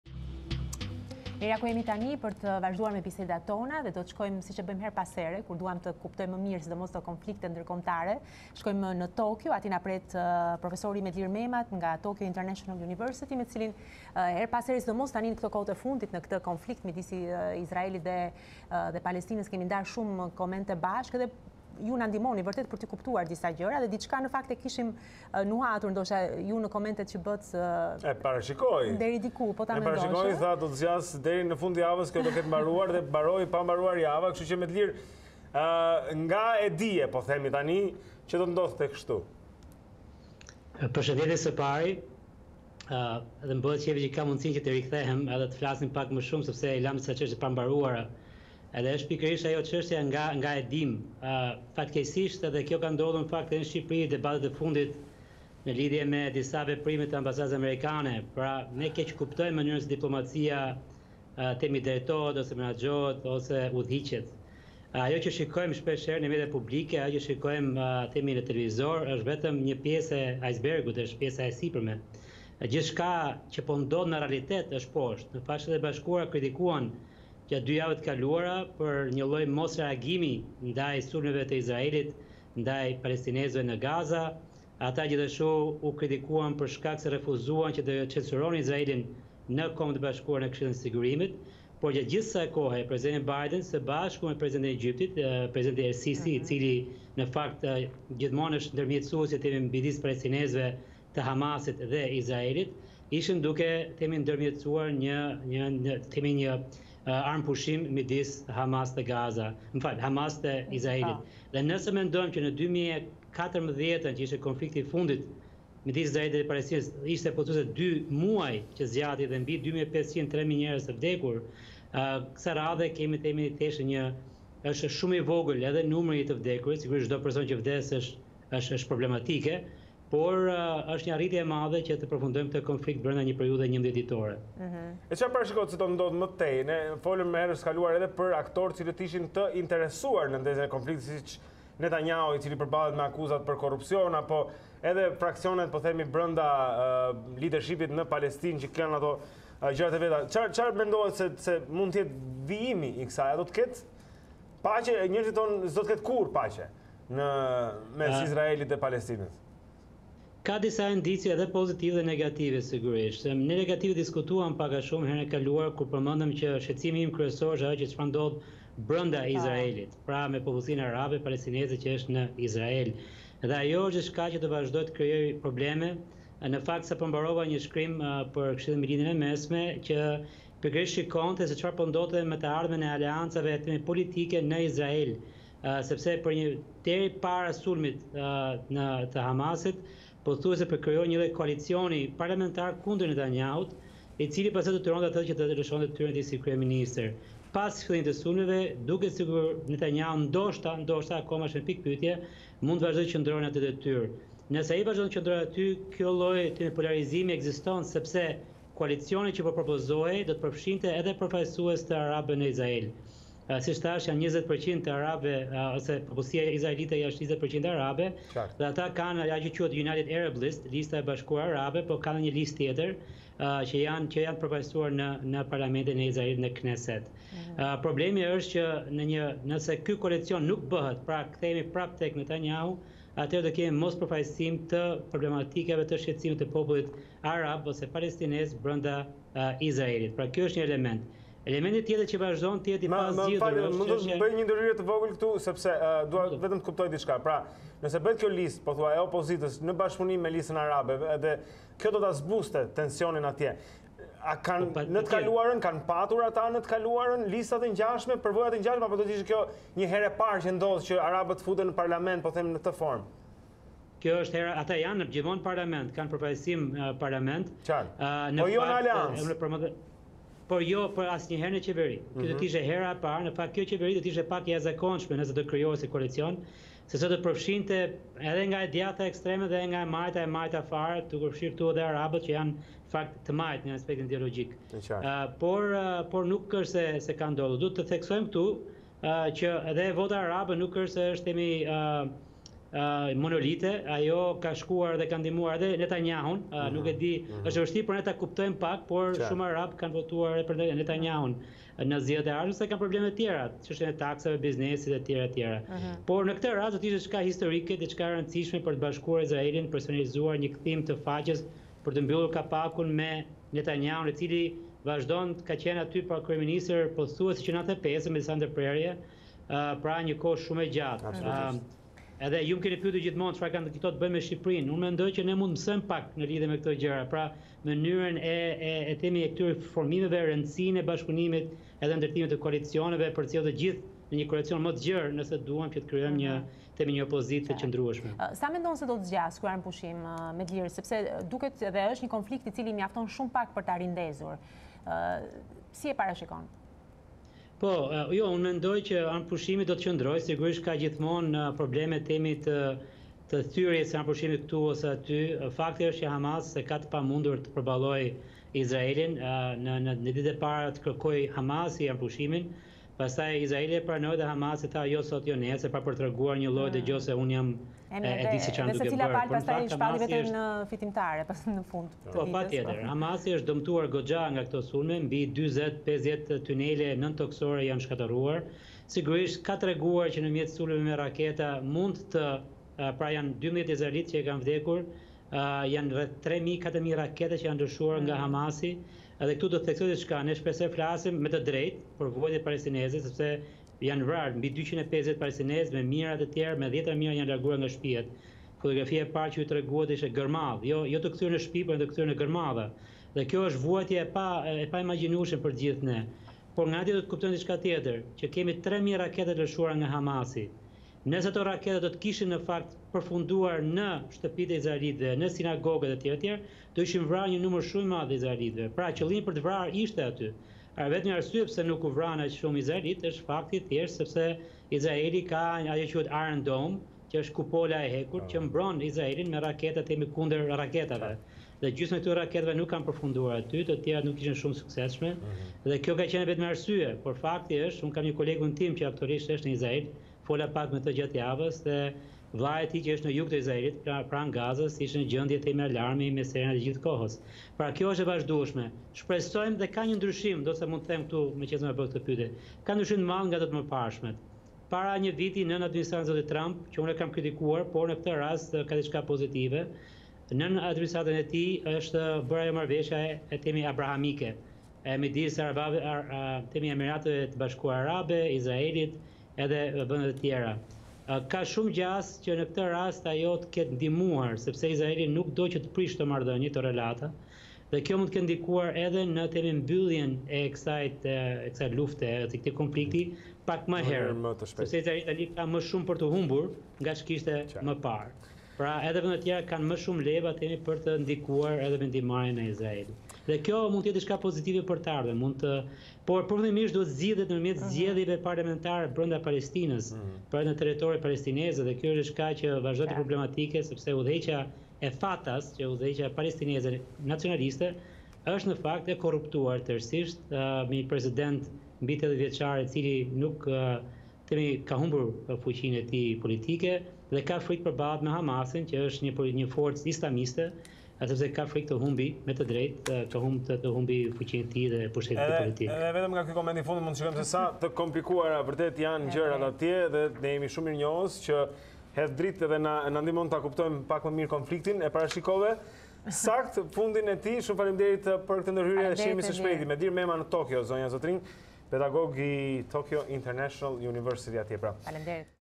Izrael-Palestinë, pas sulmeve në dyshim qëndrimi i Netanjahut në detyrë
Pedagogu në Tokio Medlir Mema komenton zhvillimet në Lindjen e Mesme